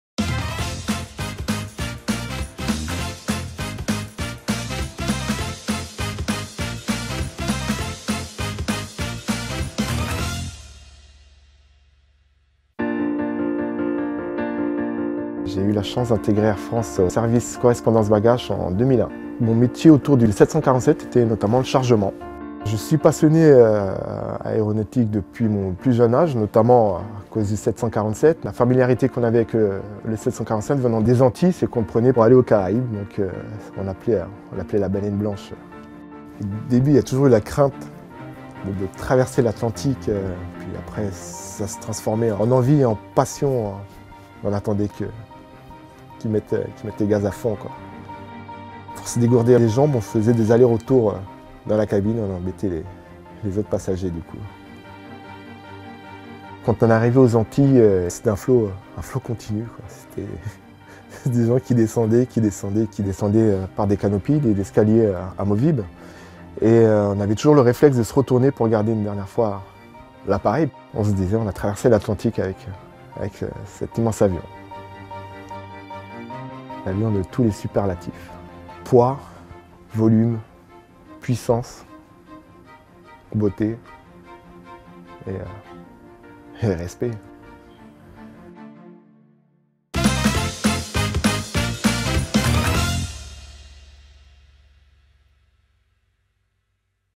Air France 747 Forever - Des voyages d'enfance et une vocation
Enfant il l’appelait la « baleine blanche », adulte il travaillera sur ceux d’Air France.